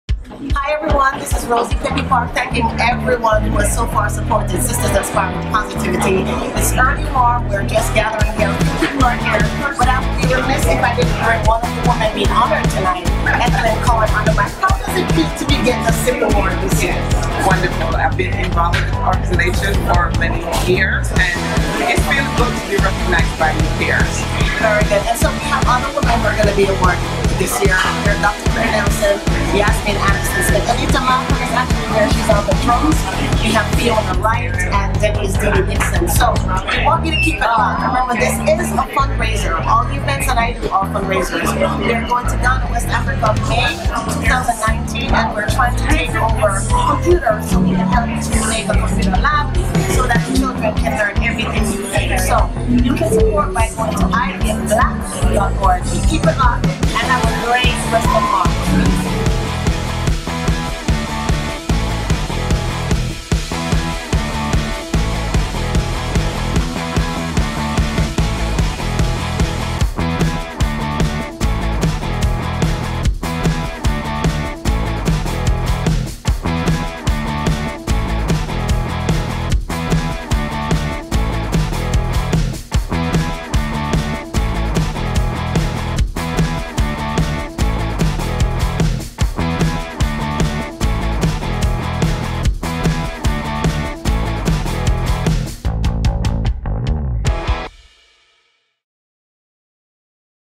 Sistahs Inspiring Postivity (SIP) - Highlights
On Saturday, March, 30, 2019, the 14th Annual Women's HerStory Month event, Sistahs Inspiring Positvity (SIP) was held.  This event is produced and hosted by Rosie Peppy Parke, @Mspeppy2U and proceeds raised are used to support